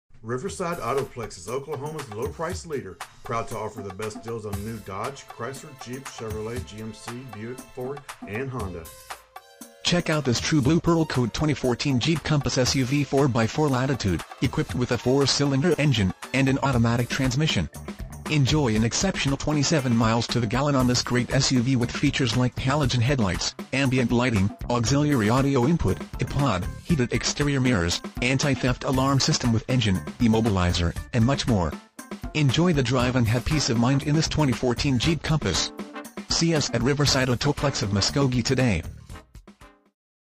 2014 Jeep Compass Riverside Autoplex of Muskogee Muskogee, OK 74401
Jeep Compass Imagine driving this True Blue Pearl Coat Jeep Compass SUV 4X4, equipped with a 4 Cylinder engine and an automatic transmission. Enjoy an exceptional 27 miles to the gallon on this great SUV with features like Halogen Headlights, Ambient Lighting, Auxiliary Audio Input - Ipod, Heated Exterior Mirrors, Anti-Theft Alarm System With Engine Immobilizer, Electronic Brakeforce Distribution, Front Fog Lights, Speed Sensitive Central Locking Power Door Locks, Emergency Braking Assist, Heated Drivers Seat, Full Time Four Wheel Drive, and much more. Enjoy the drive and have peace of mind in this Jeep Compass. See us at Riverside Autoplex of Muskogee today!
MILES:     28
VIN:1C4NJDEB1ED734652

1711 West Shawnee
Muskogee, Oklahoma 74401